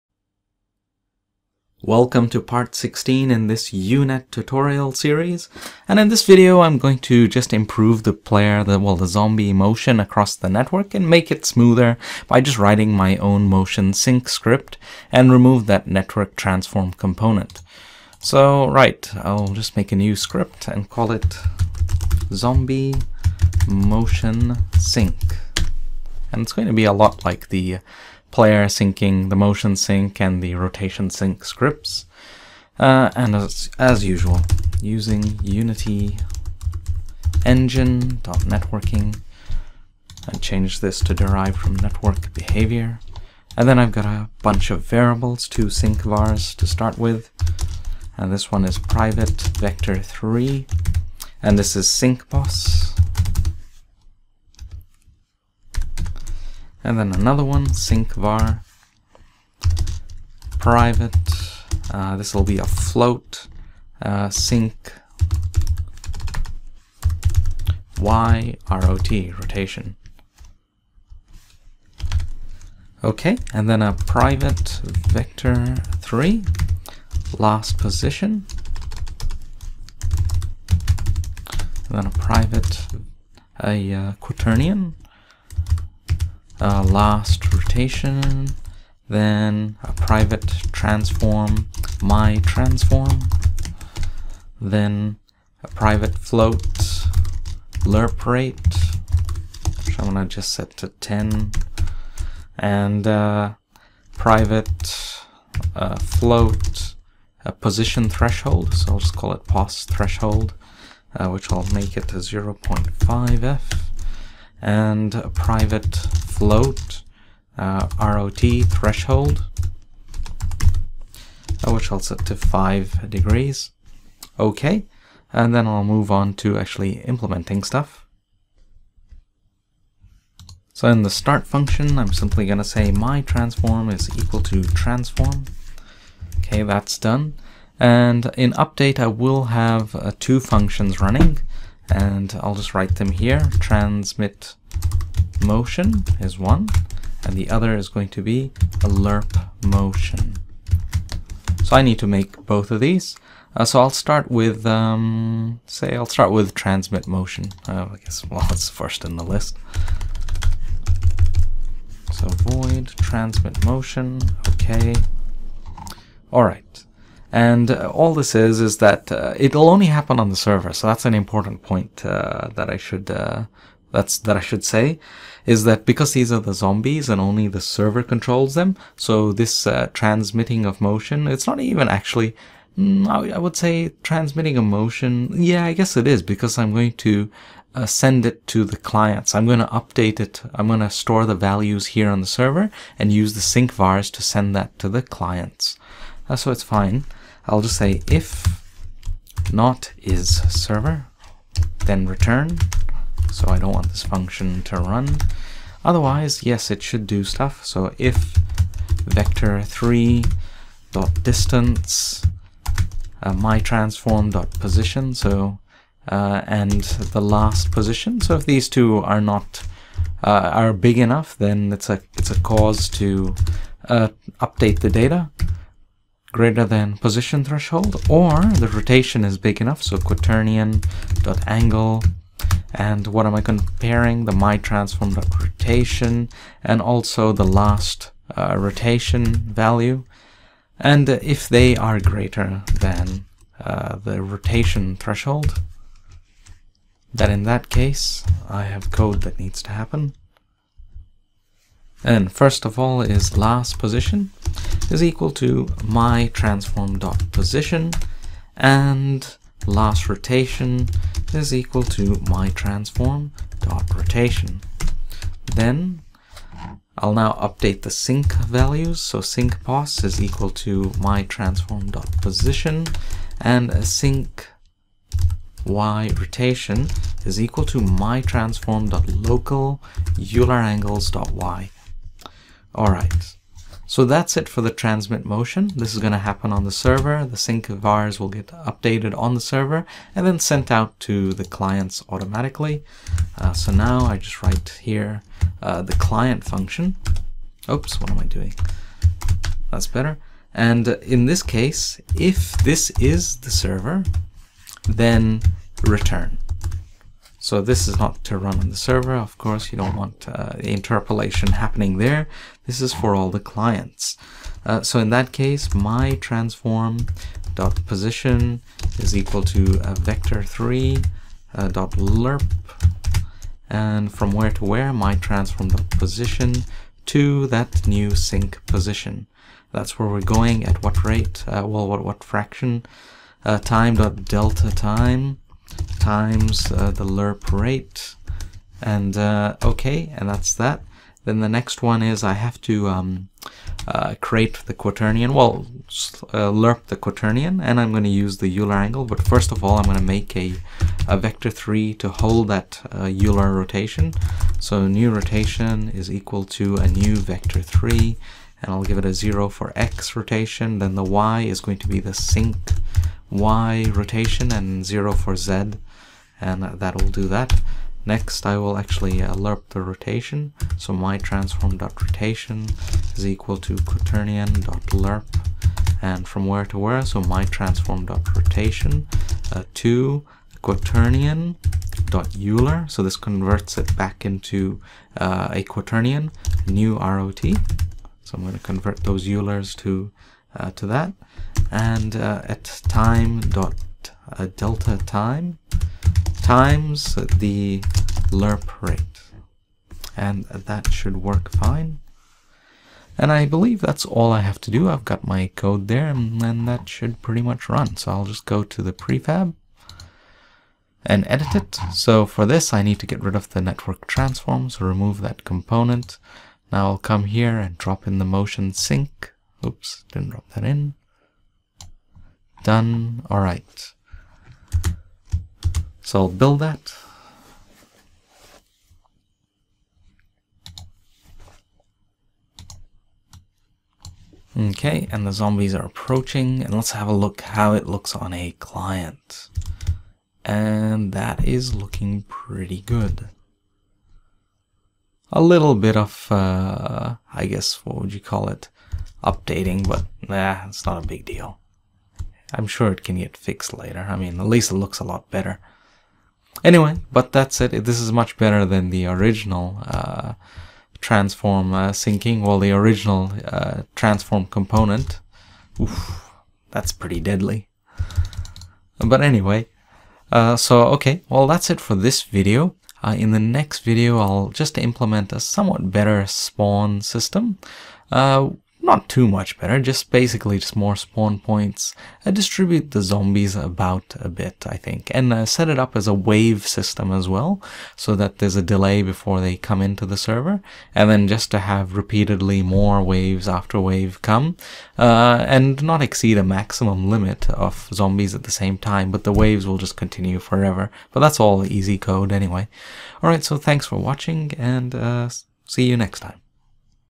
UNET Part 16 - Zombie Motion Sync
Time to remove that jittery motion over the network.